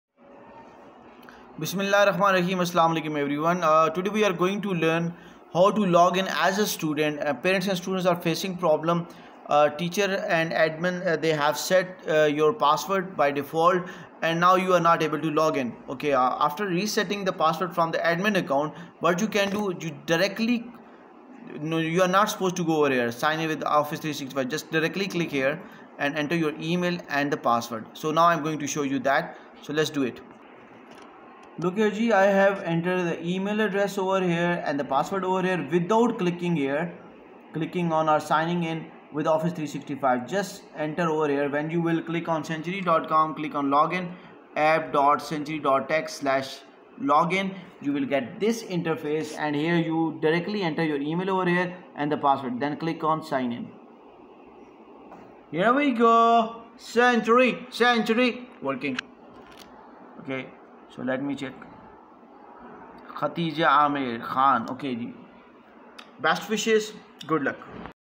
How to login in Century without Signing in Office 365
without using office 365
how to login century directly 2025-26